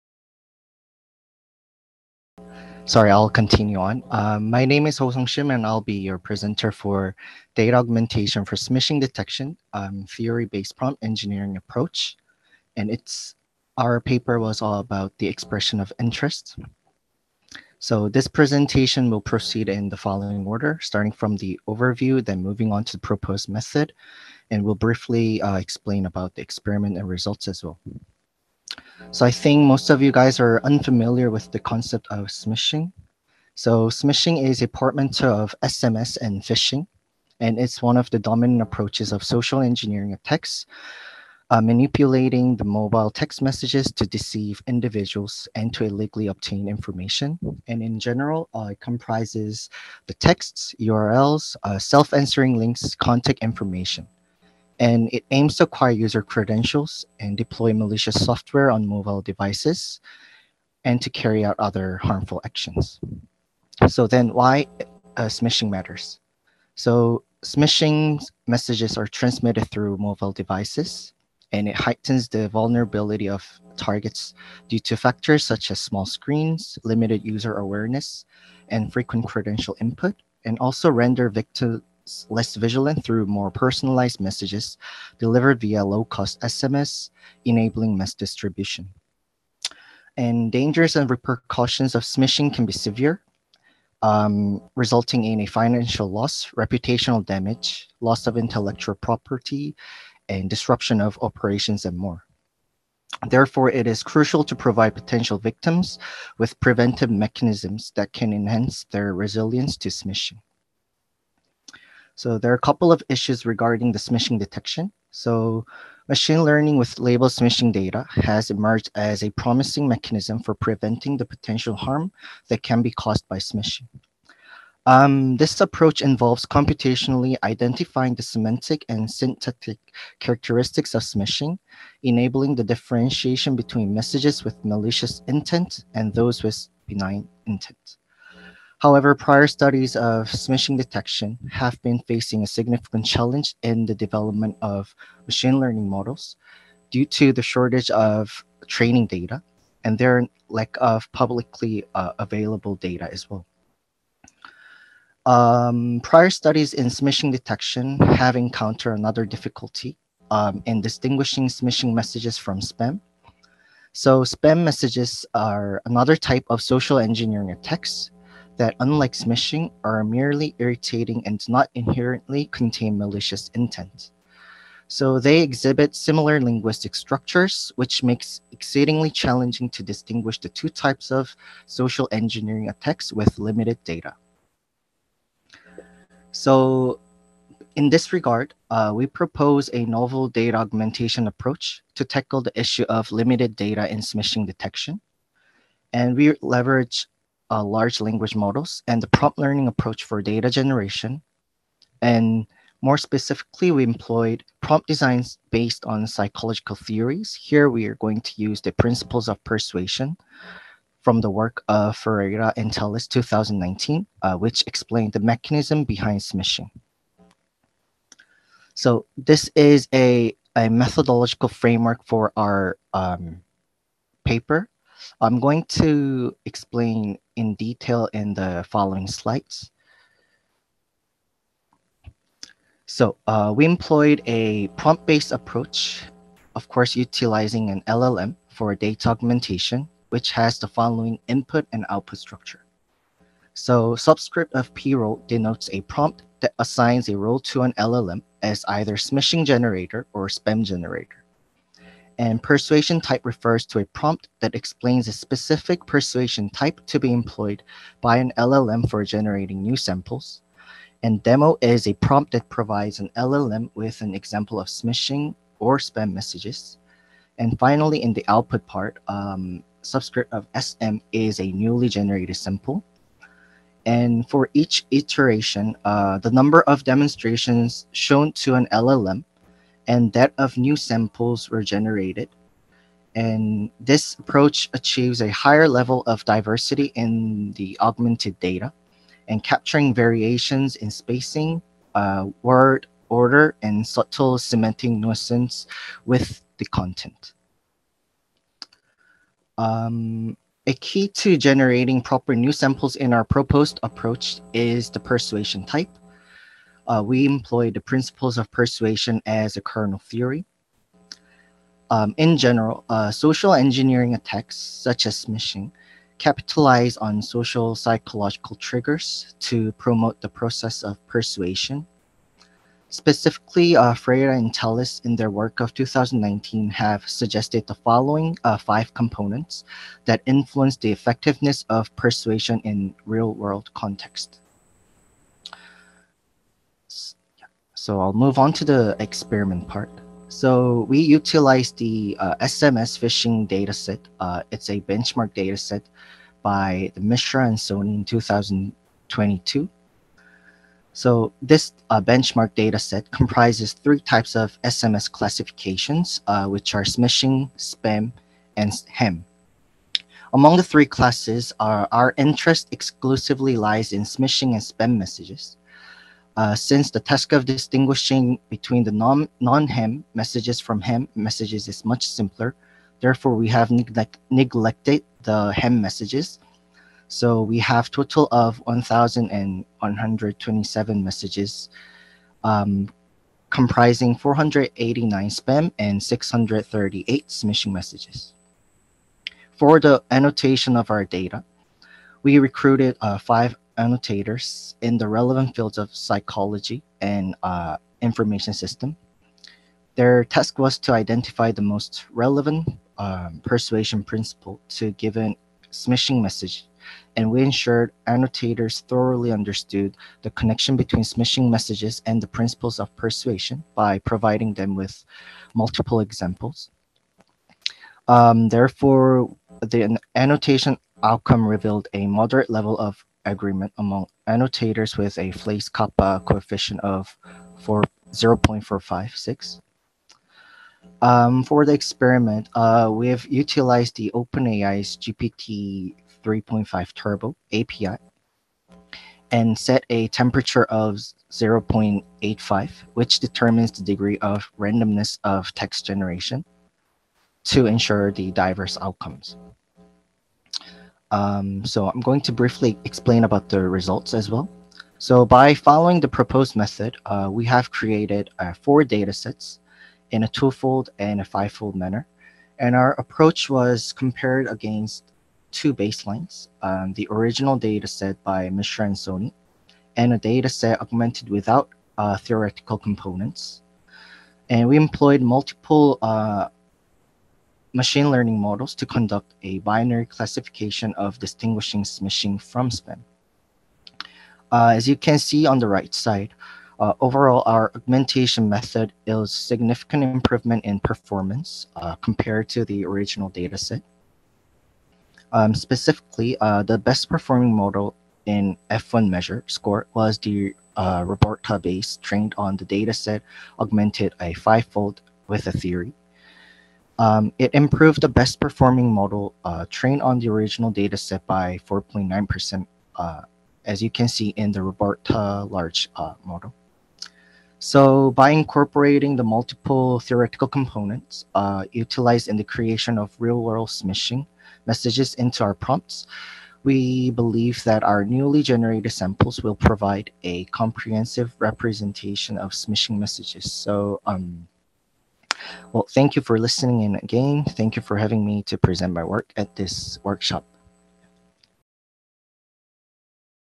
Data Augmentation for Smishing Detection A Theory based Prompt Engineering Approach
Ho Sung Shim, Information Systems, Korea University Business School, Seoul, Republic of Korea